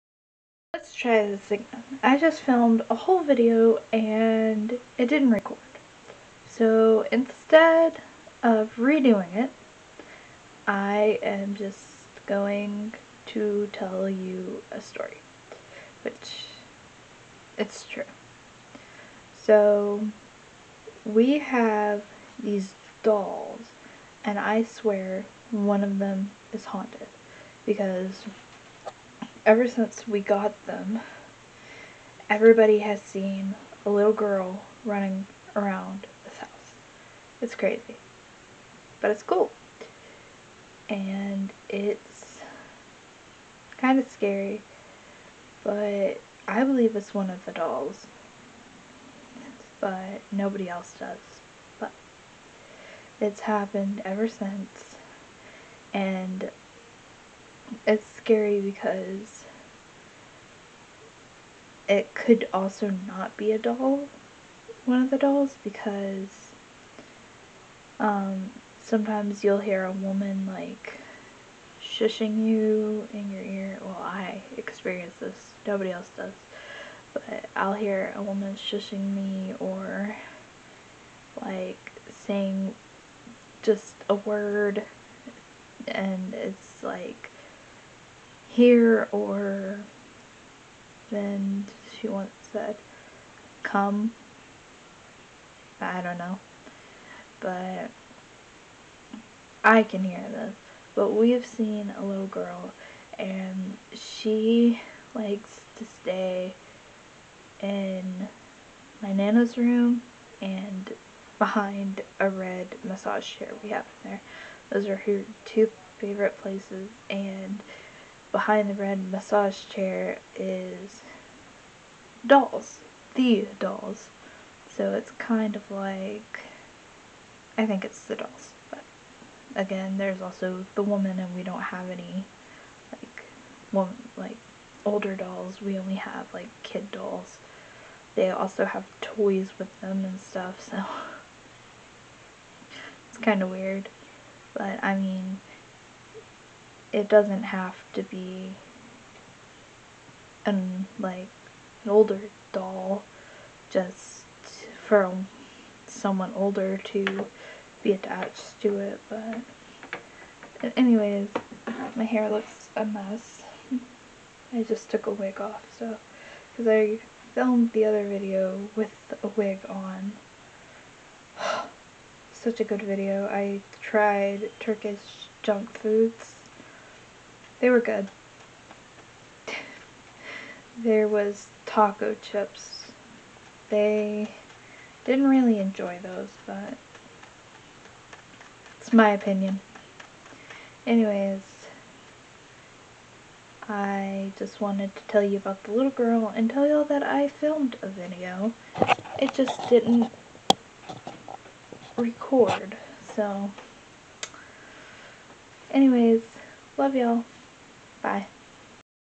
the little girl
I filmed a whole video of me trying food as I said in the video, but the dang thing didn't record. so I decided to tell you about the little girl and the women who haunts my nanas house.  Hope you enjoy.
                                                                                               ~Midnight~